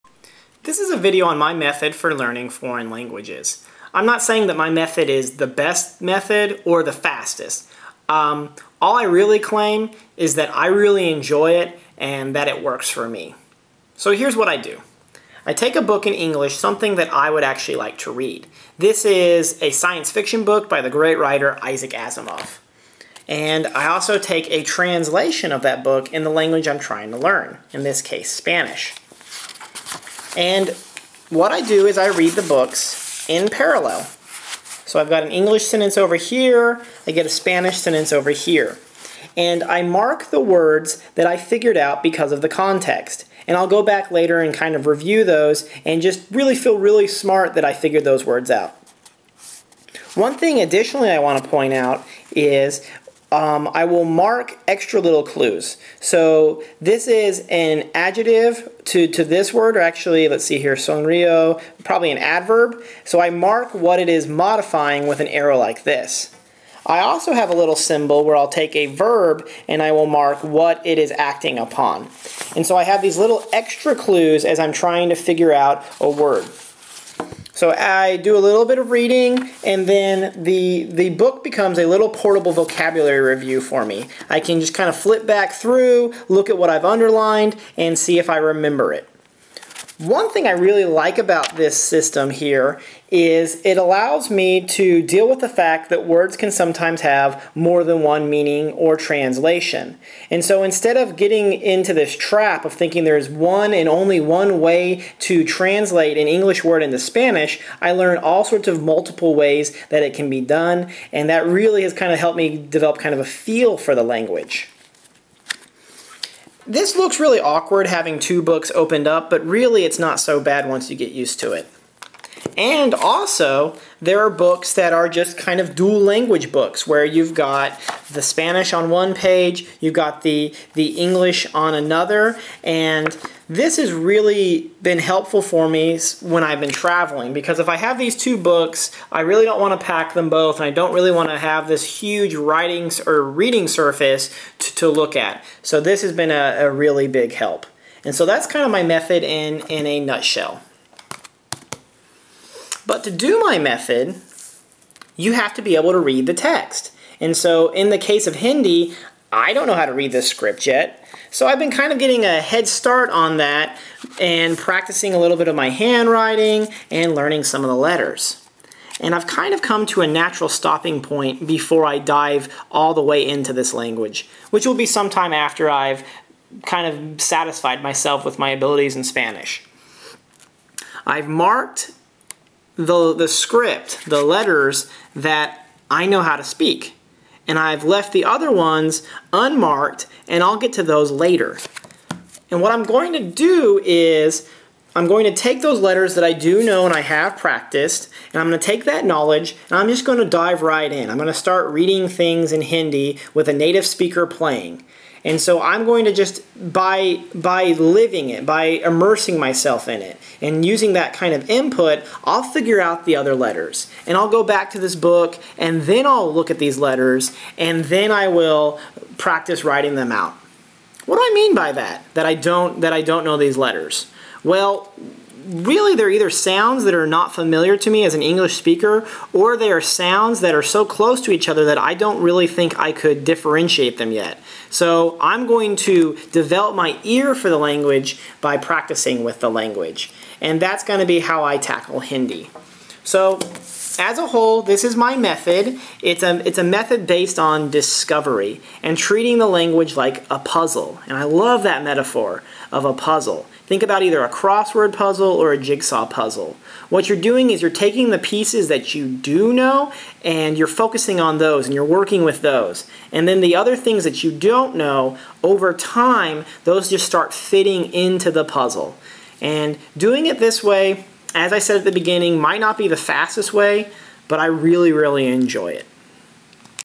My Method for Learning a Language
My method for learning a language is based on getting a lot of input in a language reading interesting books with English and the other language side by side.  After this I might use, other, more traditional methods.